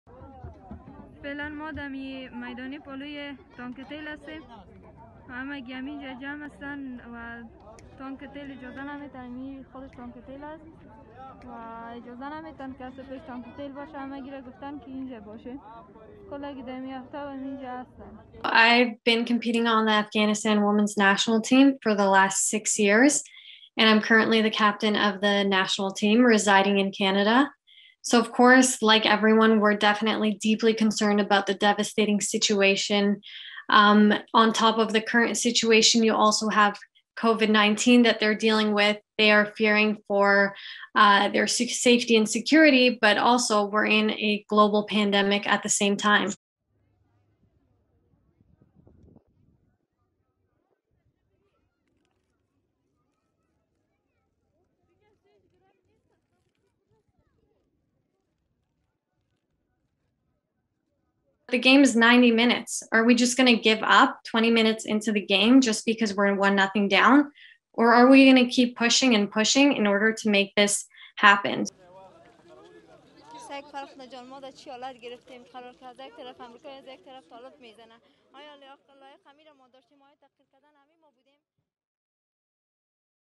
Effort continues to rescue remaining members of girls' football team in Afghanistan
There have been at least five failed attempts to rescue the girls in recent days as they were moved around for their safety. Most members of the Afghan women's team were evacuated to Australia last week.